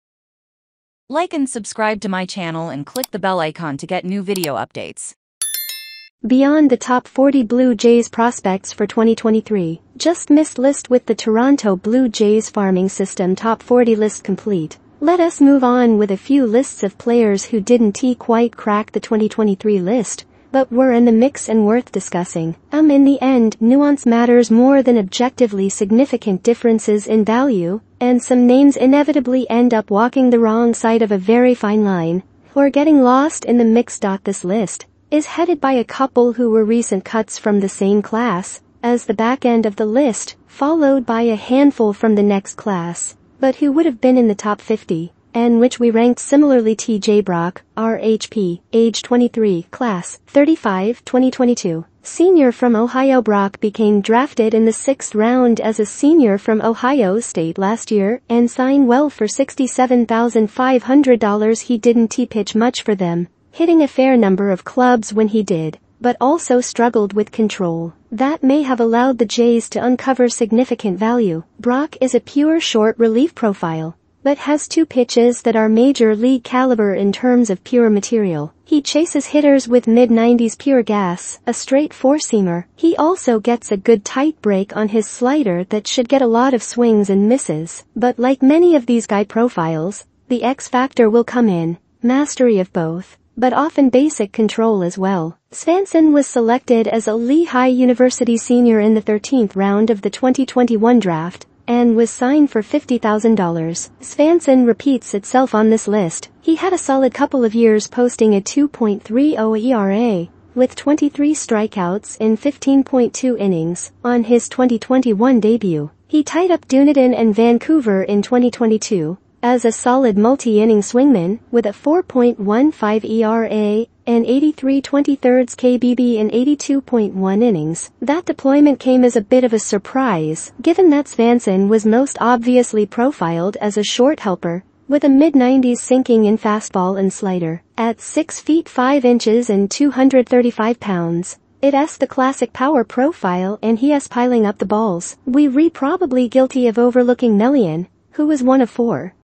BEYOND THE TOP 40 BLUE JAYS PROSPECTS FOR 2023: JUST MISSED LIST
TJ Brock. RHP. Age 23. Class: 35. 2022: Ohio State Senior Were probably guilty of overlooking Melean. who retired from Venezuela as one of four players in the 2017 international class and a bonus of over $750.000. This may be another instance where circumstances may have prevented De Jesus from fulfilling his full claim. Drafted in the 28th round of the 2017 draft. Schneider has slowly moved up the system.